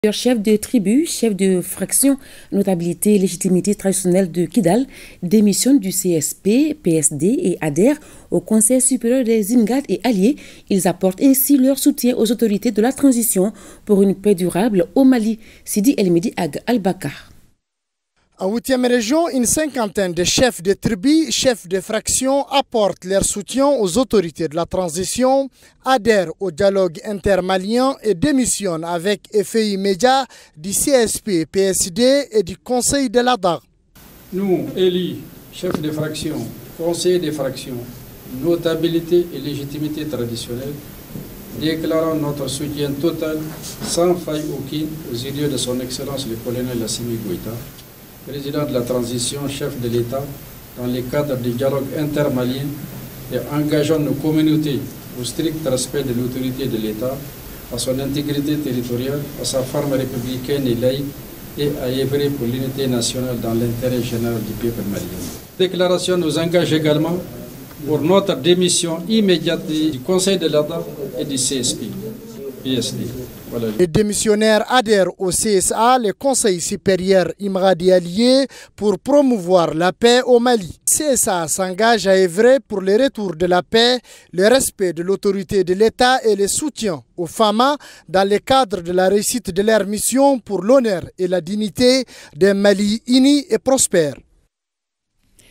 ❤️KIDAL🇲🇱: La FIN de CSP suite à plus de 50 démissions Pour le bien de notre Peuple.
Source : ORTM ; ORTM-KIDAL